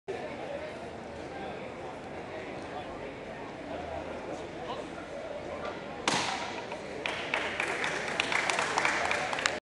Maxi Rotura en Examen Para cinturon Negro de Taekwondo ITF
roturas